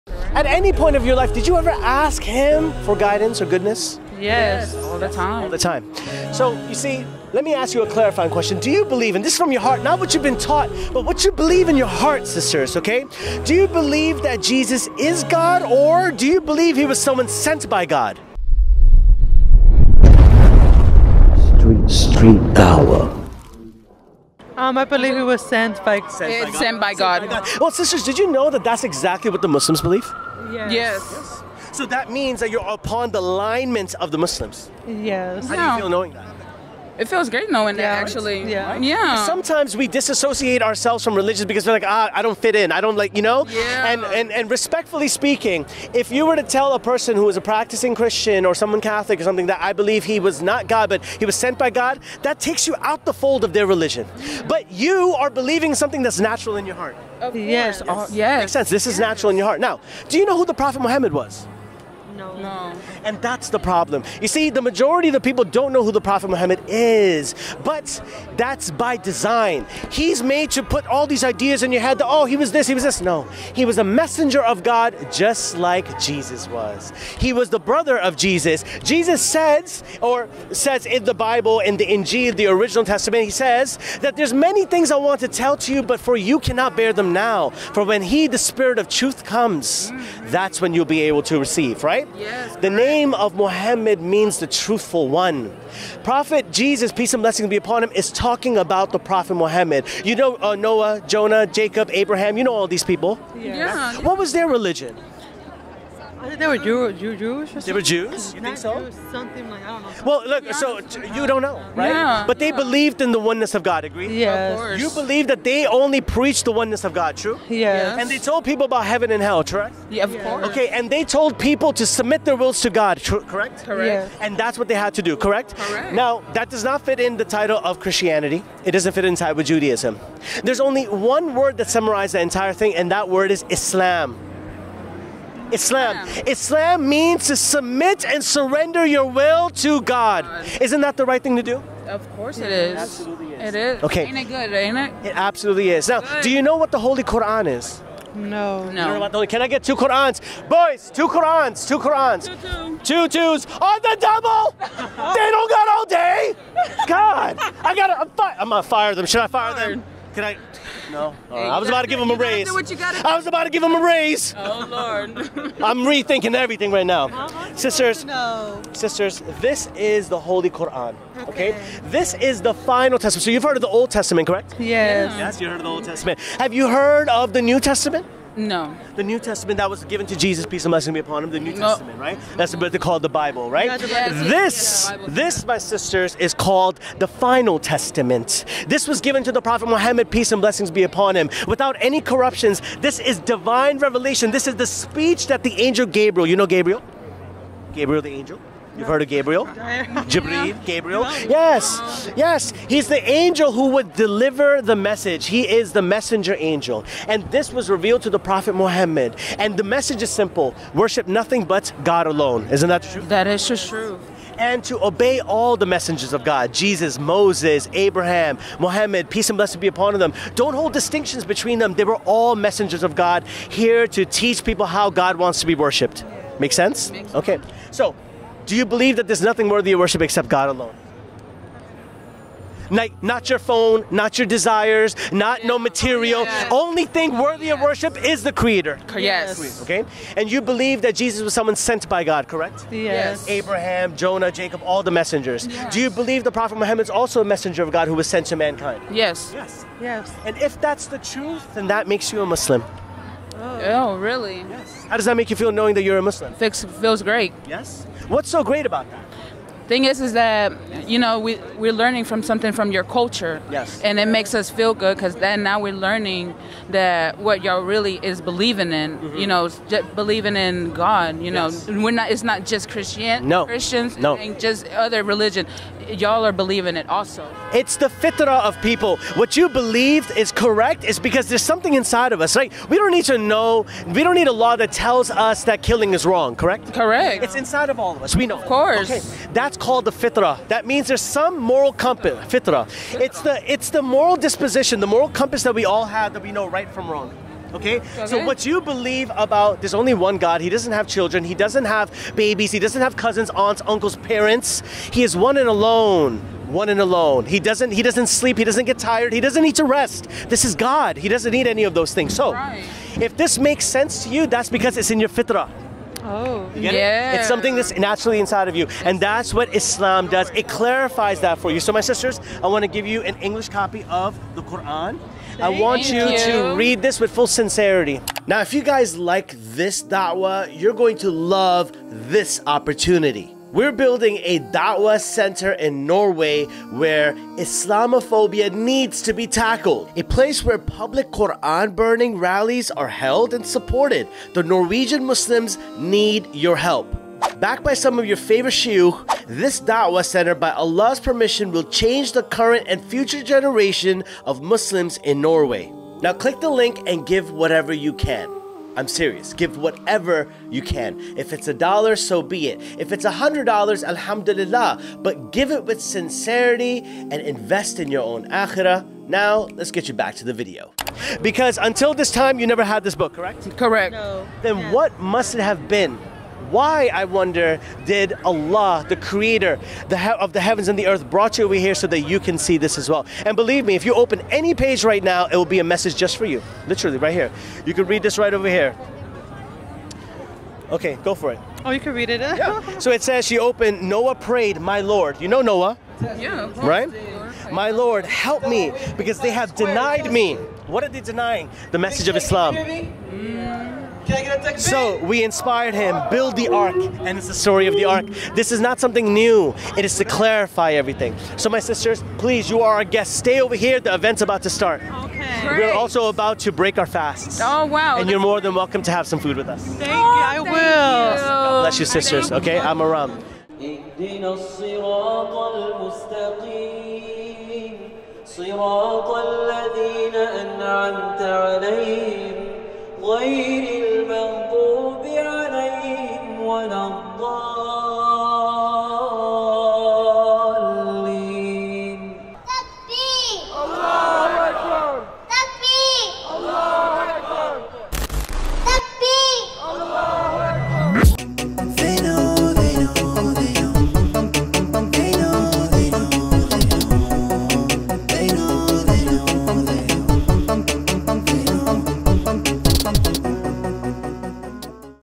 TWO Women CHALLENGED The Quran! | STREET DAWAH
These TWO women are SHOCKED by The Quran! STREET DAWAH